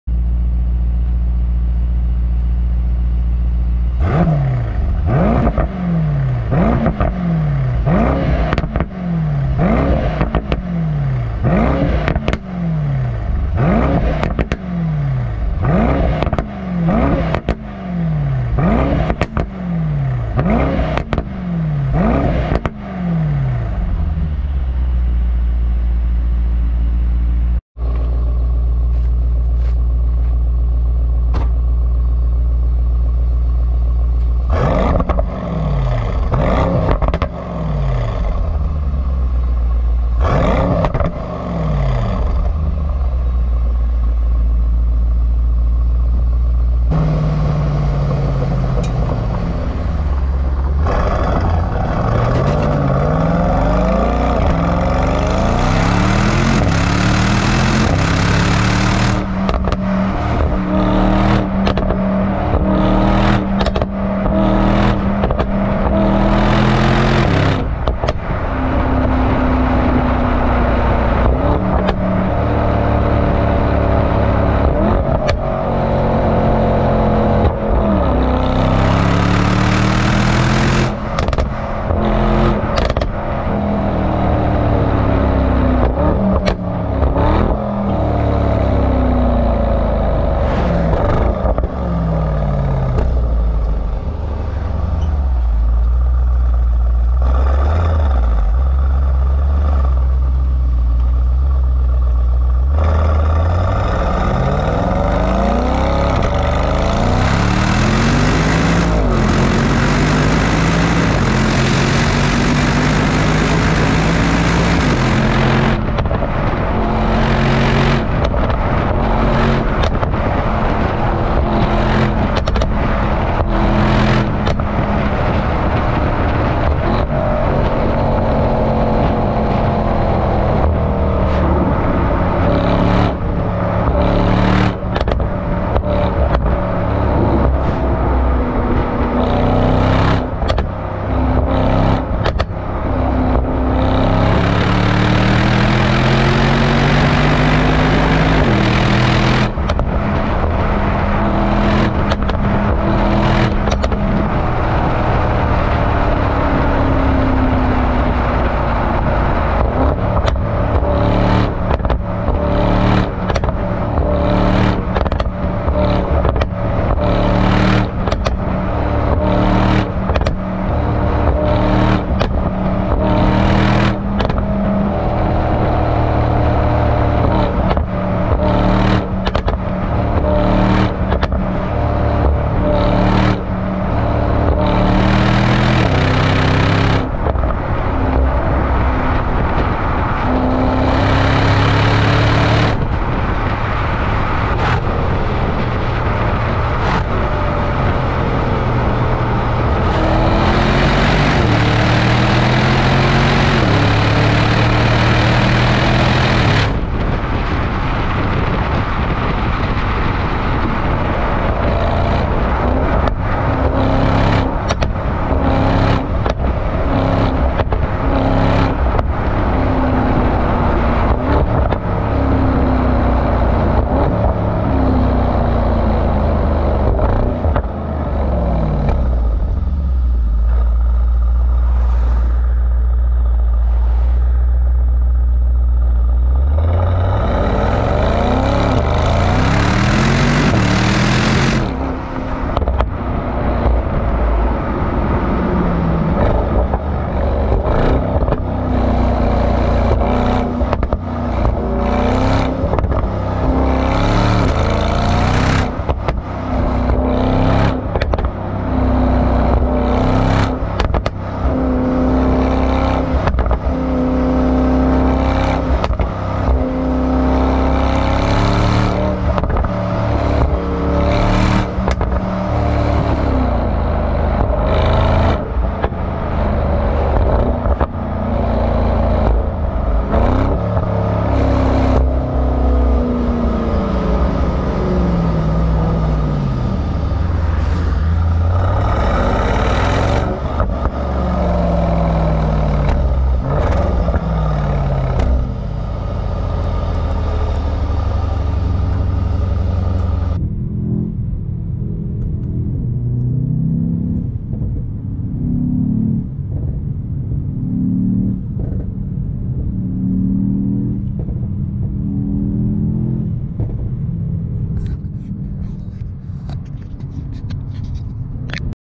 MERCEDES BENZ E63S 4.0 V8 W213 AMG SOUND CHECK EXHAUST
Hello
watch our wideo with MERCEDES S63S AMG biturbo sound check
4.0 v8